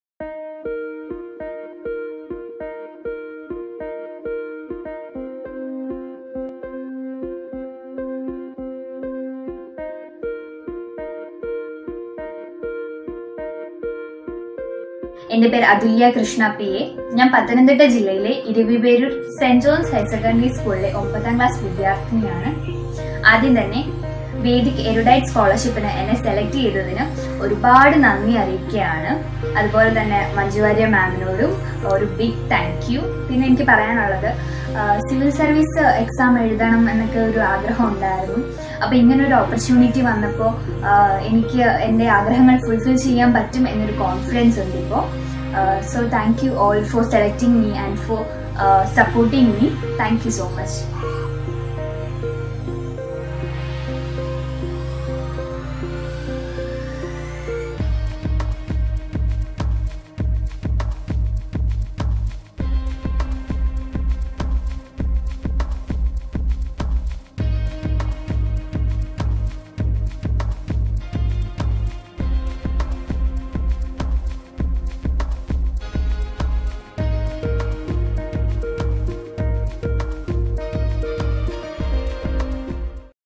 Adhulya Krishna P.A, a class IX student of St. John’s HSS, Eraviperoor, Tiruvalla
Adhulya expresses her gratitude for the chance she got to pursue her career dream. She thanks Ms Manju Warrier, Actor, for enabling this through the Vedhik Erudite Scholarship.